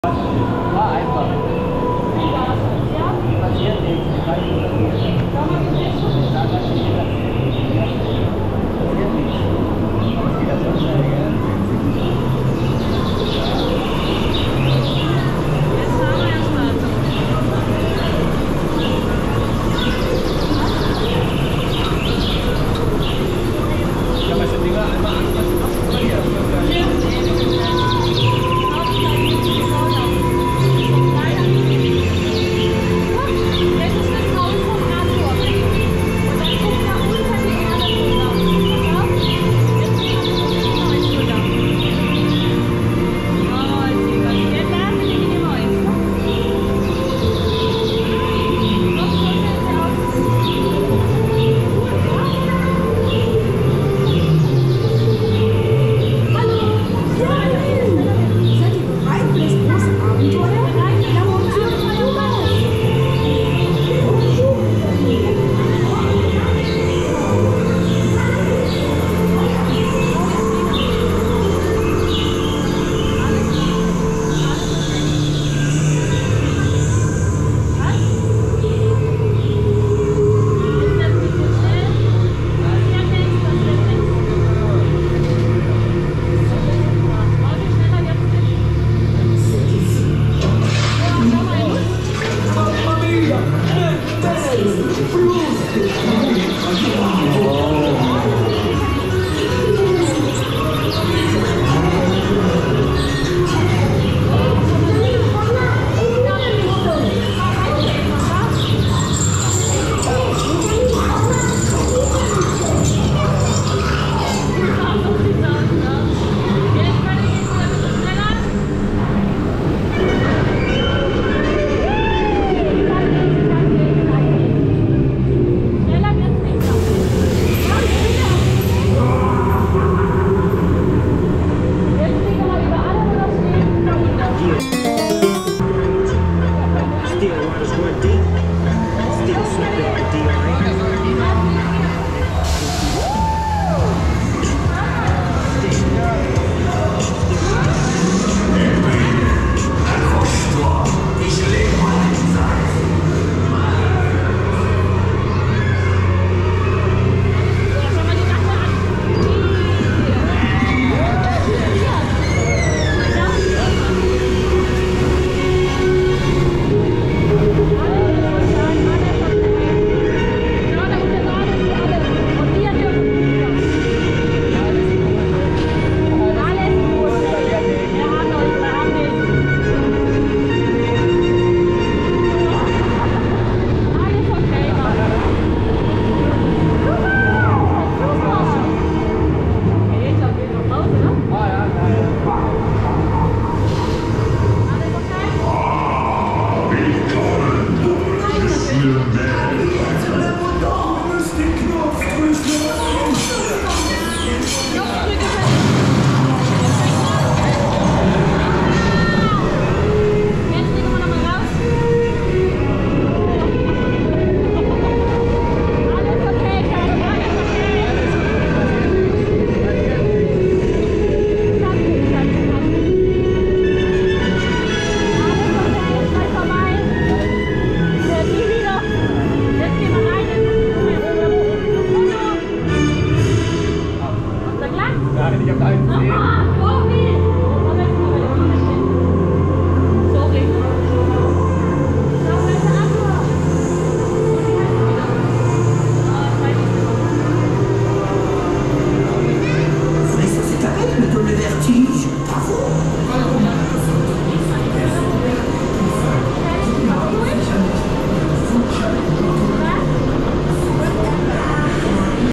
Arthur POV (2024, 4K 60FPS), Europa Park Mack Inverted Powered Coaster/Dark Ride | Non-Copyright
Arthur was the prototype inverted powered coaster from Mack.  Located at Europa Park, this innovative ride system combines elements of both a dark ride and roller coaster.

The following was added to bleep out a racial slur during the ride's onride audio: